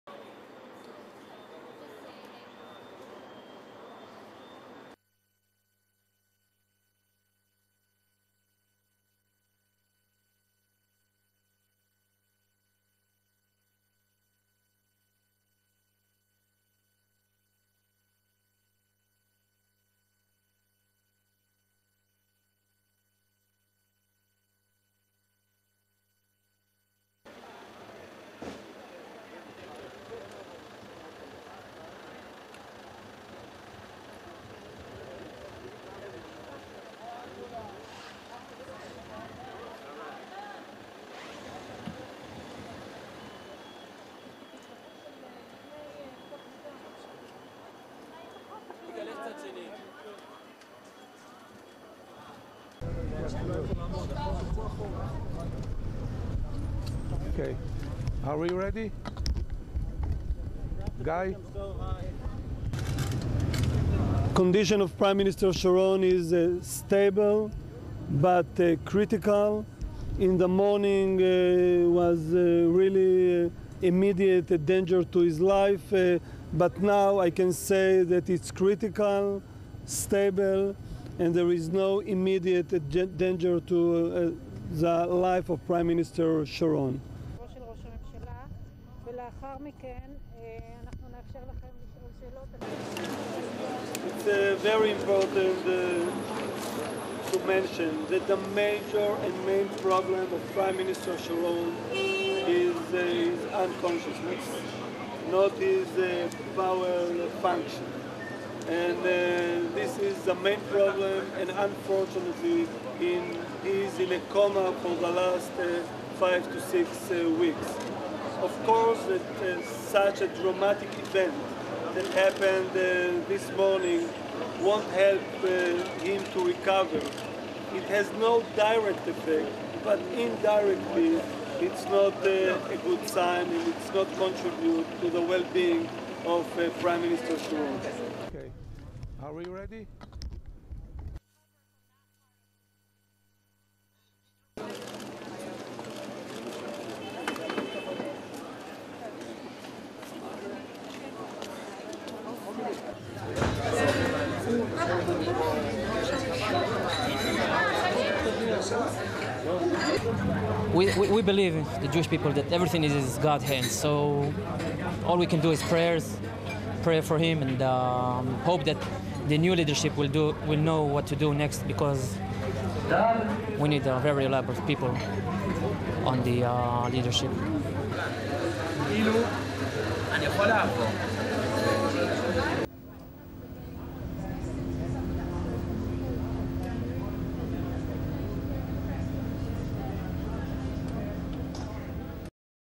WRAP Omri, Sharon's adviser visit, latest briefing, voxpop, hospital
1. Wide of Hadassah Hospital, where Ariel Sharon is being cared for
2. Sharon's son Omri arriving at hospital
3. Yal Arad, one of Sharon's closest advisors who ran Kadima campaign, arriving by taxi
4. Hadassah Hospital director Shlomo Mor-Yosef walking to greet journalists
5. SOUNDBITE: (English) Shlomo Mor-Yosef, Hadassah Hospital director:
"The condition of Prime Minister Sharon is stable but critical, in the morning, there was really an immediate danger to his life, but now I can say that it's critical, stable, and there is no immediate danger to the life of Prime Minister Sharon."
6. Wide of media briefing
7. SOUNDBITE: (English) Shlomo Mor-Yosef, Hadassah Hospital director:
"It very important to mention that the major and main problem of Prime Minister Sharon is his unconsciousness, not his body function. And this is the main problem. Unfortunately he is in a coma for the last five to six weeks. Of course such a dramatic event that happened this morning won't help him to recover. It has no direct effect, but indirectly, it's not a good sign, and it's not going to contribute to the well-being of Prime Minister Sharon."
8. Wide of briefing
9. Wide of Shlomo Mor-Yosef leaving news conference, pan to hospital
10. Mid shot of Jerusalem residents sitting in coffee shop watching hospital briefing
11. SOUNDBITE: (English) Haim Daniel, Jewish resident of Jerusalem:
"We believe, the Jewish people, that everything is in God's hands so, all we can do is prayers, pray for him, and hope that the new leadership will know what to do next. We need very elaborate people on the leadership."
12. Mid shot of coffee shop
13. Exterior shot of hospital at night
STORYLINE:
Doctors removed one-third of Ariel Sharon's large intestine in four-hour emergency surgery on
Saturday, the Israeli prime minister's seventh operation since he suffered a massive stroke and slipped into a coma five weeks ago.
The latest complication further dimmed already slim hopes of any recovery.
At 1500 GMT, Hadassah Hospital's director announced that Sharon's surgery was over and that he had been returned to the hospital's intensive care unit.
Doctor Shlomo Mor-Yosef said the surgery was a relatively simple procedure and that Sharon's main medical problem continues to be his coma.
"Of course such a dramatic event that happened this morning won't help him to recover. It has no direct effect, but indirectly, it's not a good sign, and it's not going to contribute to the well-being of Prime Minister Sharon," Mor-Yosef said.
The hospital director said Saturday's deterioration slims Sharon's chance of recovery, and increases risks of additional illnesses.
A hospital spokesperson said earlier on Saturday that an abdominal scan showed that blood was not reaching parts of his intestines and that his digestive tract had suffered severe damage.
Sharon's sons, Omri and Gilad, rushed to the hospital as their father's condition worsened.
Sharon's stroke jolted Israel, which is gearing up for an election on 28 March.
The centrist Kadima Party's strong lead in the poles hasn't been rattled by Sharon's hospitalisation and sudden departure from politics.
In a Jerusalem caf�, Israelis closely followed their 77-year-old leader's latest ordeal, but many have already come to terms with his departure from politics.
"All we can do is prayers, pray for him, and hope that the new leadership will know what to do next," said Haim Daniel as he watched the hospital news conference in the caf�.